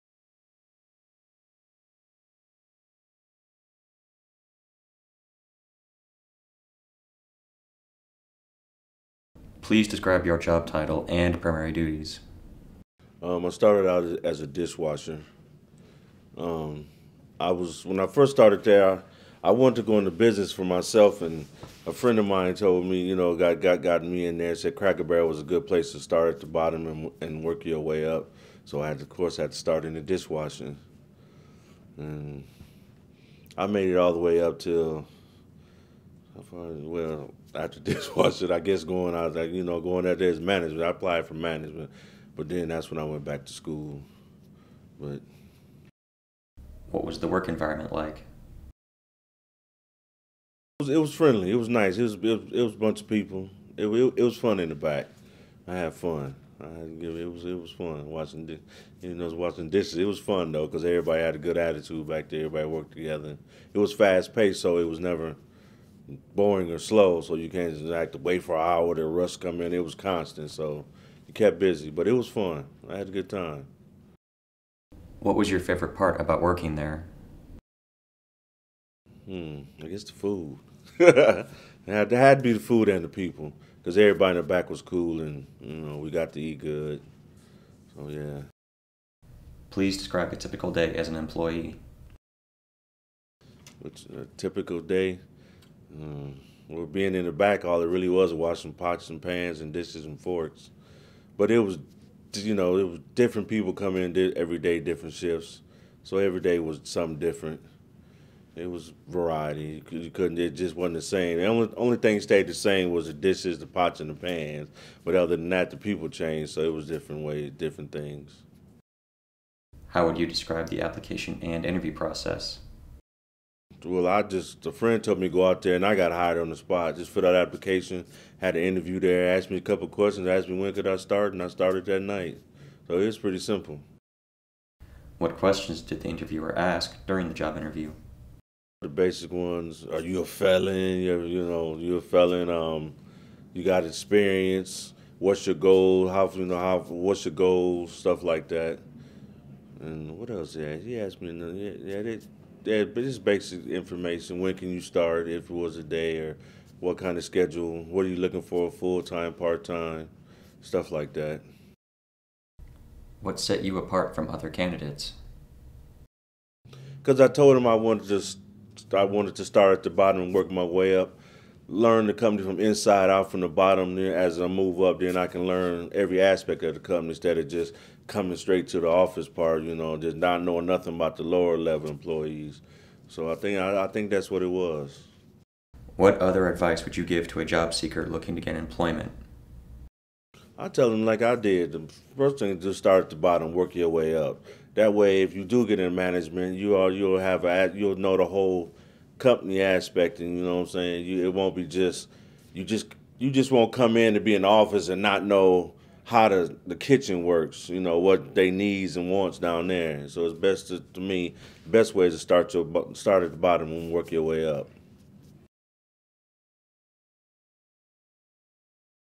Cracker Barrel Interview - Dishwasher
A former Cracker Barrel dishwasher talks about the interview process, interview questions, how to get a job and what its like to work for Cracker Barrel.